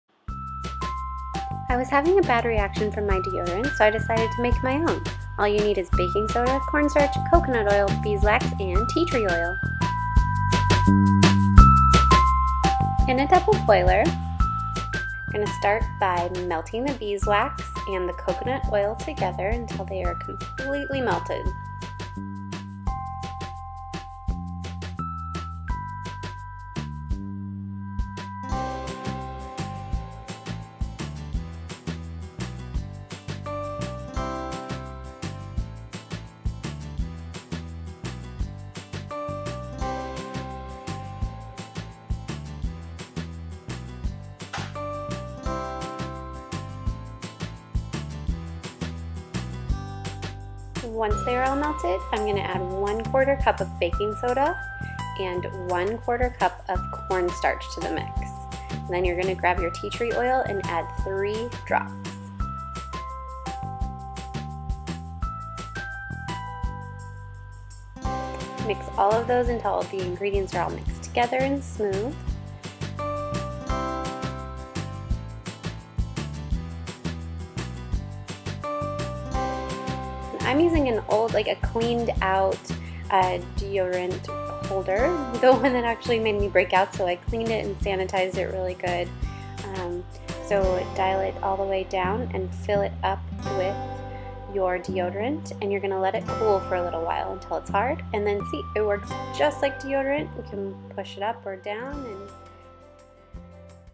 DIY Natural Deodorant
My skin wasn't loving my new deodorant so I wanted to make my own. I have been transitioning to more natural products so this made perfect sense!

Ingredients:
3 drops of tea tree oil
3 tbs coconut oil
1 tbs beeswax
1/4 cup baking soda
1/4 cup corn starch

Melt beeswax and coconut oil together then add in the corn starch, baking soda, and tea tree oil. So easy!!